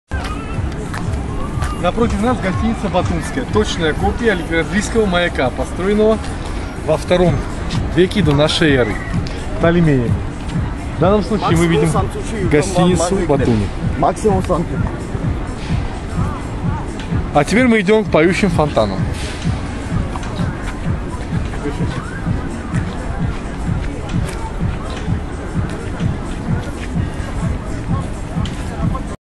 Koka Davlianidze- Ночной Батуми 2010 года..
Ночной Батуми в июле 2010 года. Романтика. კოკა-ნიკოლოზ დავლიანიძე.